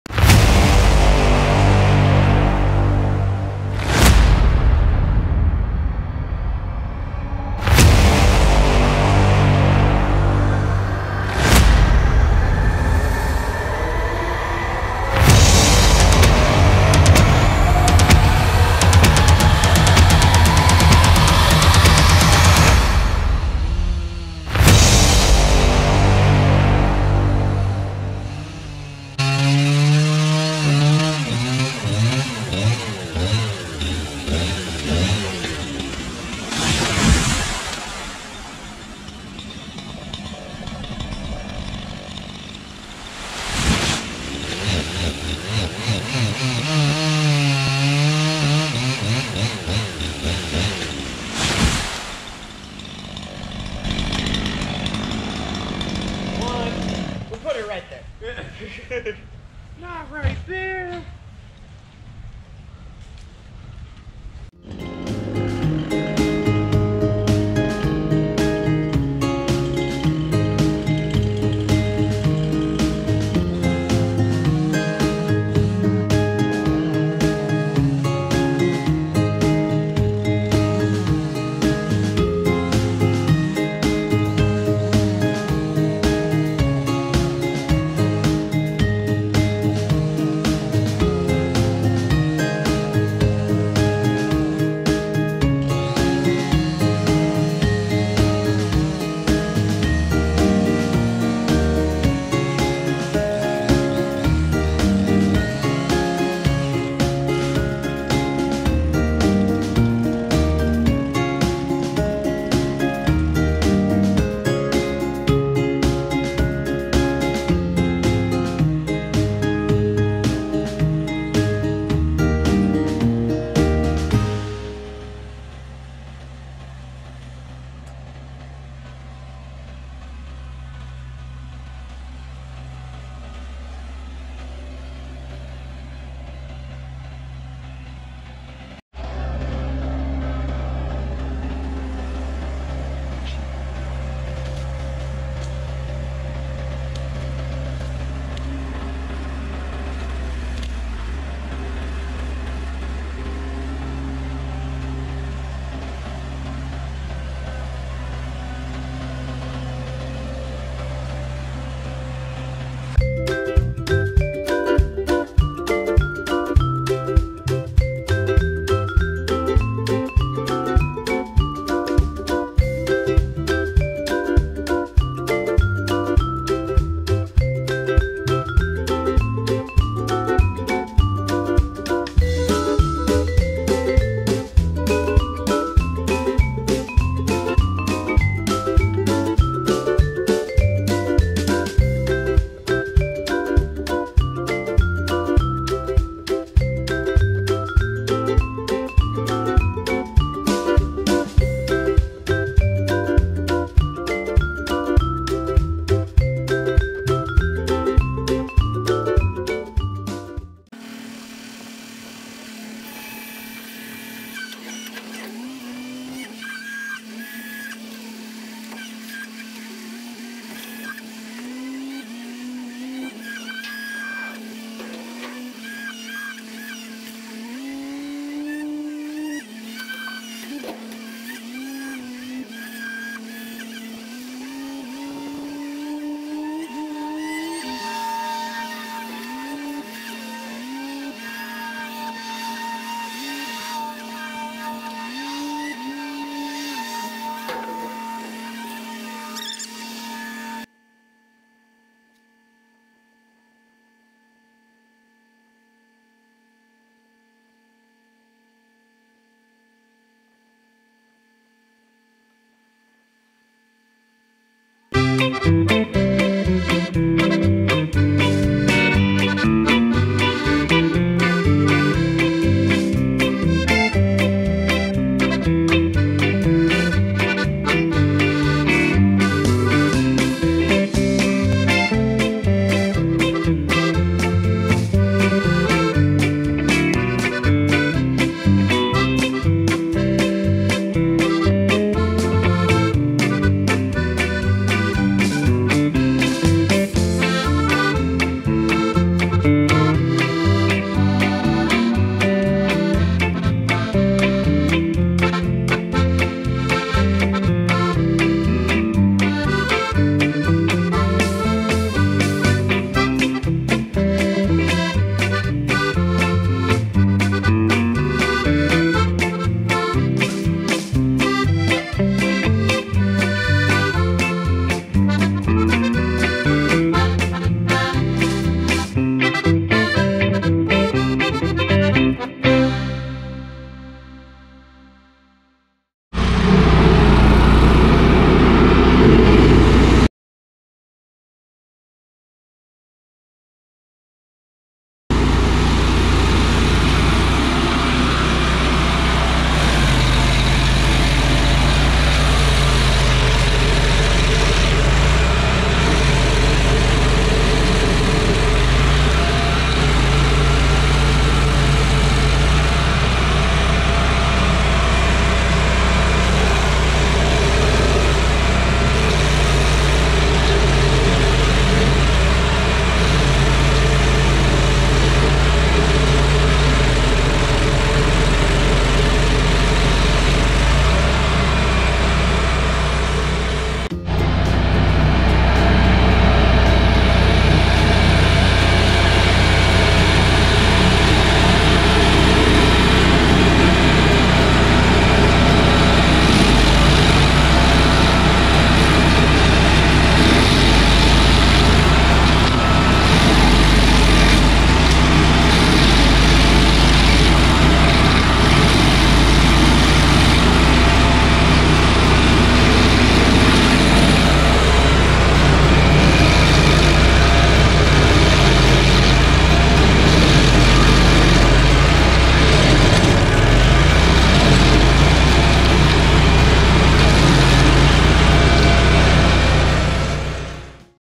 DINGO IS BACK!
WE REMOVE TWO PALMS TRIM 3 AND RAISE THE CANOPY ON SOME OAKS MAY NOT BE THE MOST EXCITING VIDEO BUT JUST LIKE SHARING WHAT WE DO!